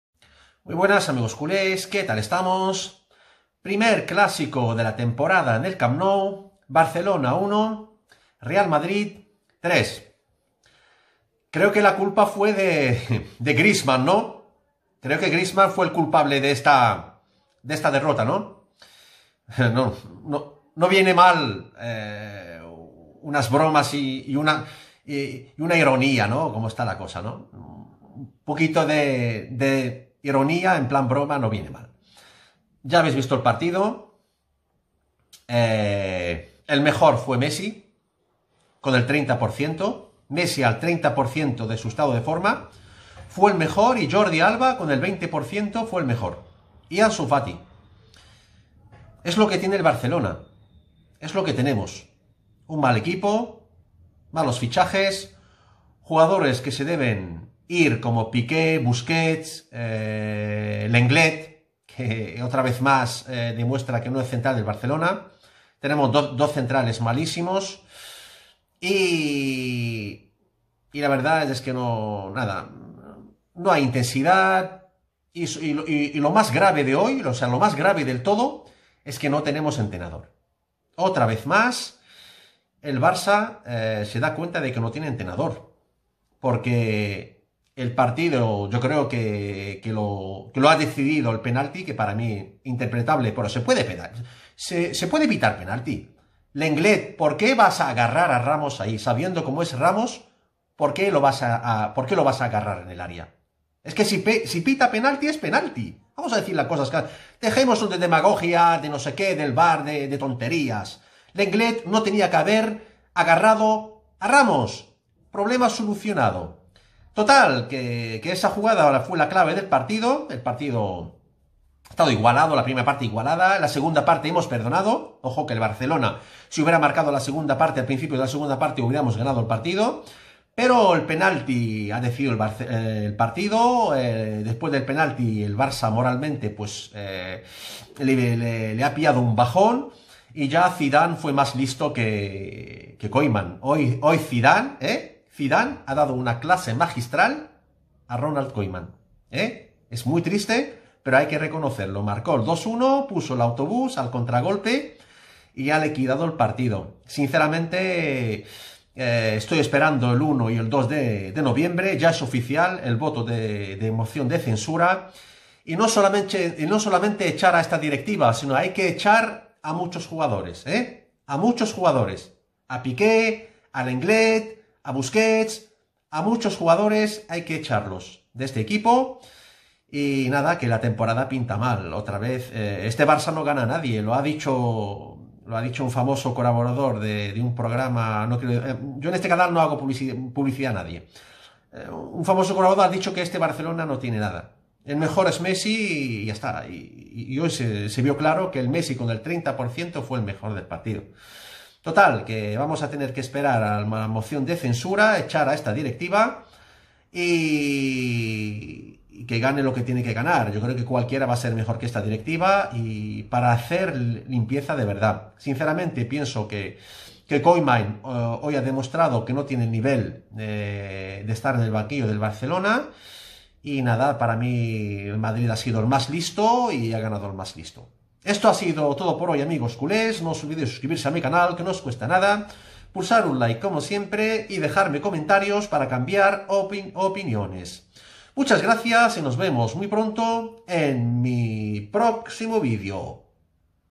BARCELONA VS REAL MADRID 1-3 | RESUMEN DEL PARTIDO | EL CLÁSICO DE LA LIGA 2020
Zidane retrata a Koeman a Bartomeu y al Barcelona en general